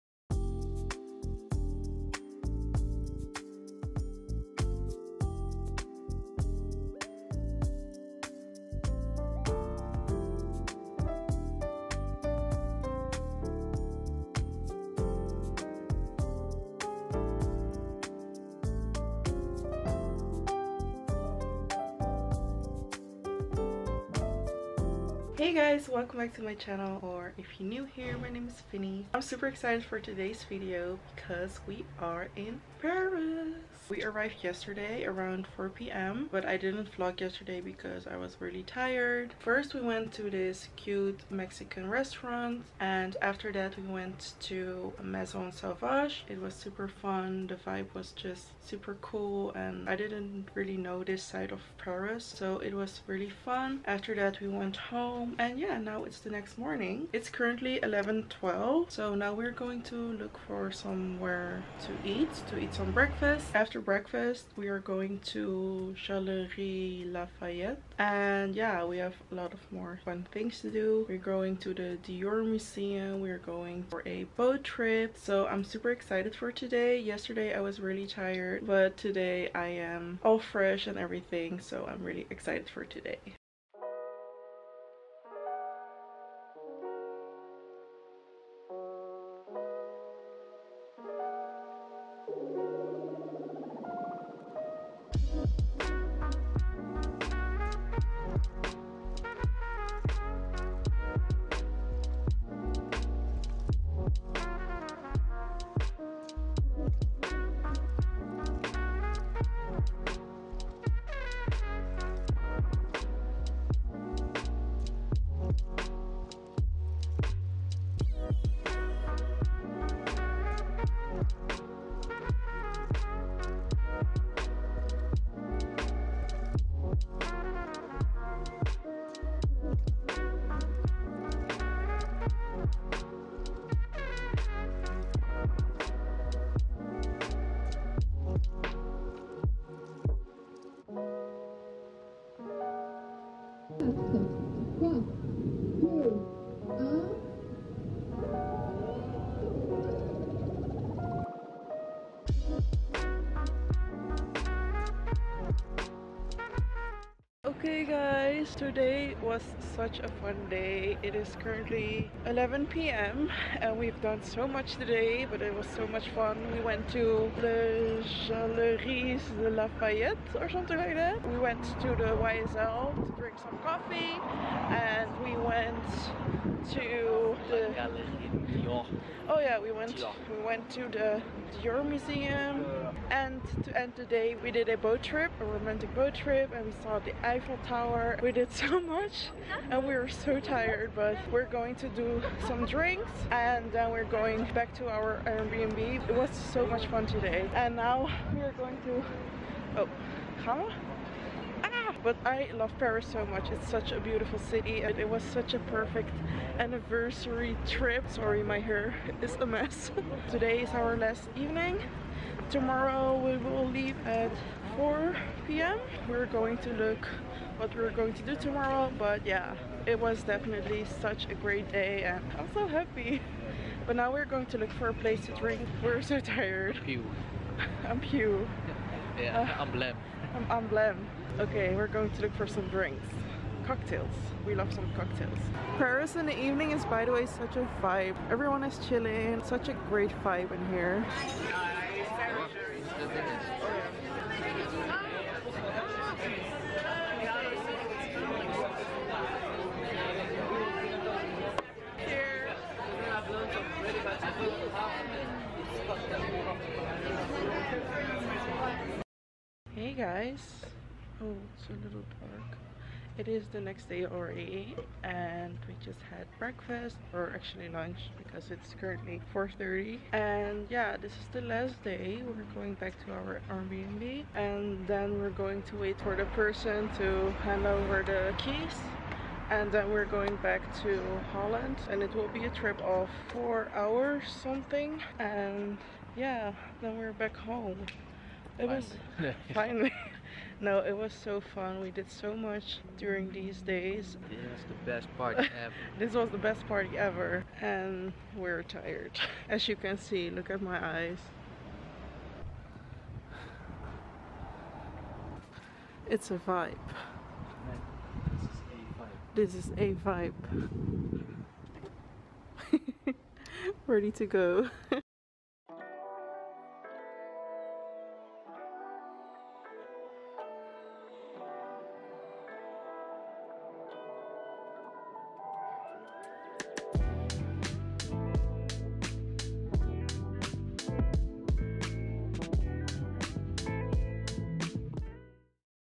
PARIS VLOG 2022 | Maison Sauvage, Saint Laurent Café, Galeries Lafayette, Galerie DIOR, Eiffel Tower
In this paris vlog 2022  I will be taking you along with me on a 3 day/anniversary trip! Celebrating our 6th anniversary in Paris ✨🇨🇵 We're visiting the popular restaurant Maison Sauvage for some cocktails, the Saint Laurent Café for coffee and we went to Galeries Lafayette Haussmann, La Galerie Dior (exhibition) and finally we did a romantic boat trip to see the Eiffel tower at night!

Spots:
Maison Sauvage Saint-Germain-des-Prés
Saint Laurent, 8 Rue du 29 Juillet
Galeries Lafayette Haussmann
La Galerie Dior, 11 Rue François 1er

MORE VIDEO'S:
WHAT I EAT IN A DAY: Healthy Apple Pie, Healthy Chicken Wraps + MORE | Easy and Healthy 2022: